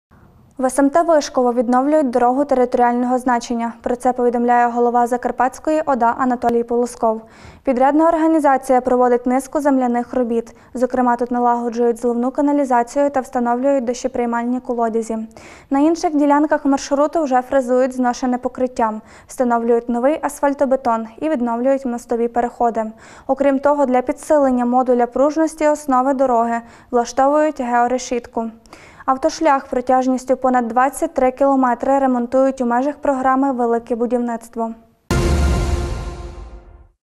Відновлюють дорогу
В смт Вишково відновлюють дорогу територіального значення. Про це повідомляє голова Закарпатської ОДА Анатолій Полосков. Підрядна організація проводить низку земляних робіт, зокрема тут налагоджують зливну каналізацію та встановлюють дощоприймальні колодязі. На інших ділянках маршруту  вже фрезерують зношене покриття, встановлюють новий асфальтобетон і відновлюють мостові переходи. Окрім цього, для підсилення модуля пружності основи дороги влаштовують георешітку. Автошлях протяжністю понад 23 км ремонтують у межах програми «Велике будівництво».